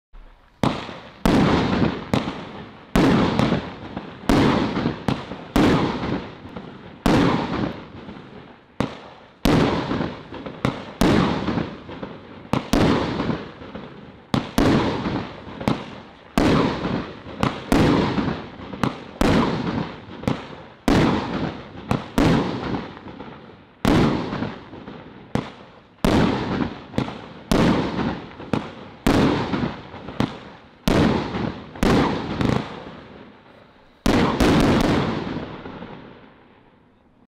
Purple Plums by Celtic from Fantastic Fireworks
Purple Plums is a very unique firework when it comes to effects. Purple dahlias burst in the sky with a red wintersweet afterglow followed by gold strobe willows with purple dahlia, then gold willow to purple with gold strobe finishing with Titanium gold coconuts with purple dahlia.